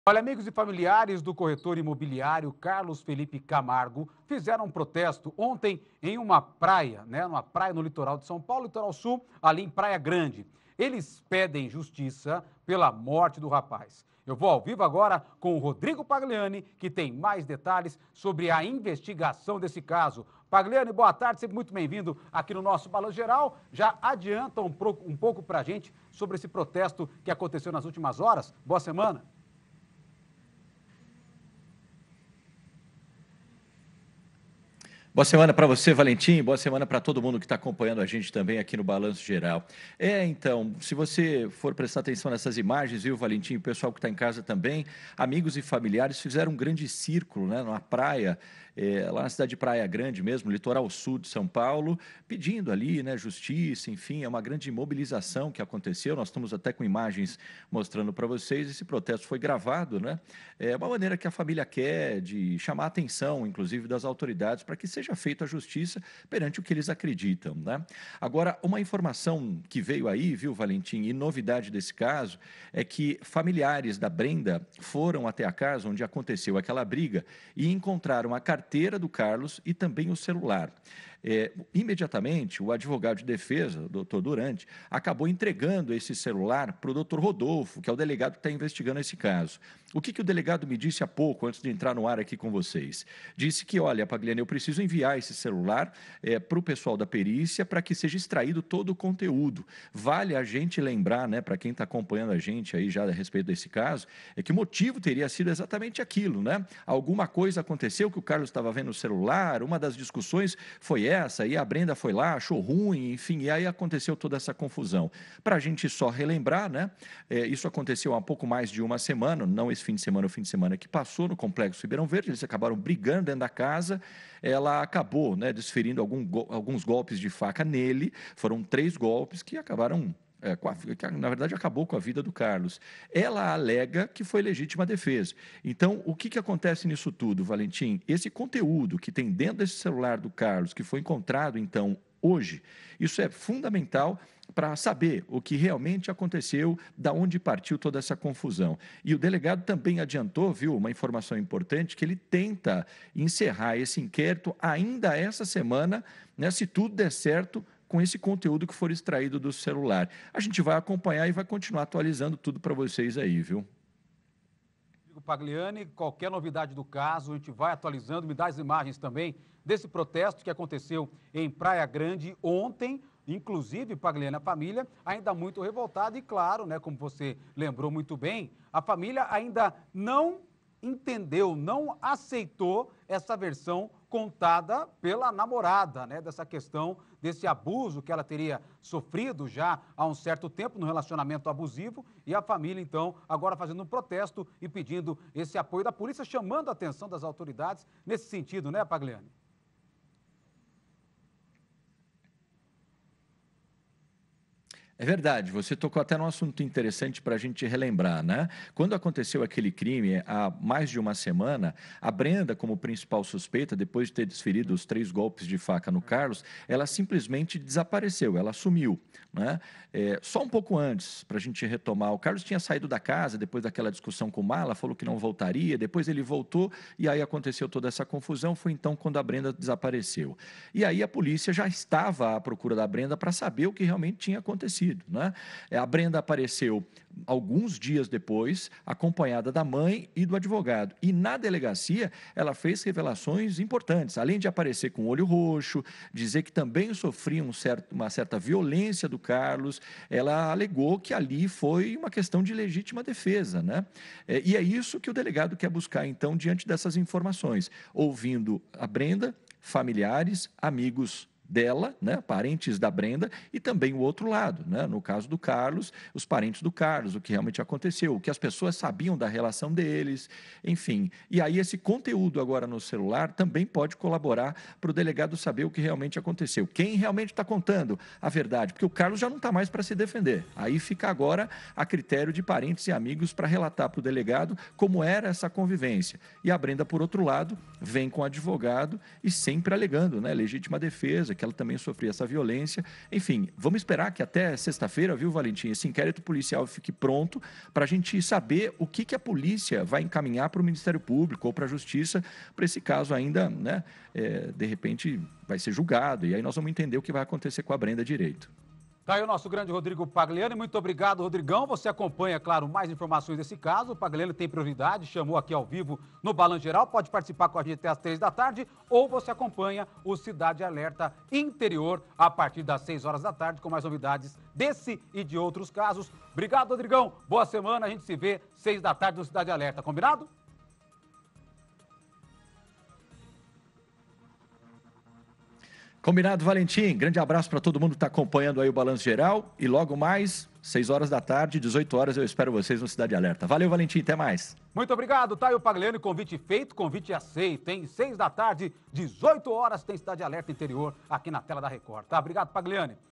Família de corretor morto pela namorada em Ribeirão Preto faz protesto no litoral de SP
Amigos e familiares do corretor imobiliário Carlos Felipe Camargo fizeram um protesto neste domingo (10), na Praia Grande, Litoral Sul de São Paulo. Eles pedem justiça pela morte do rapaz, que foi morto a facadas pela namorada Brenda Xavier, em Ribeirão Preto, no último final de semana. O celular e a carteira de Felipe foram localizados dentro da casa onde foi cometido o crime e podem ajudar a Polícia Civil nas investigações desse caso. .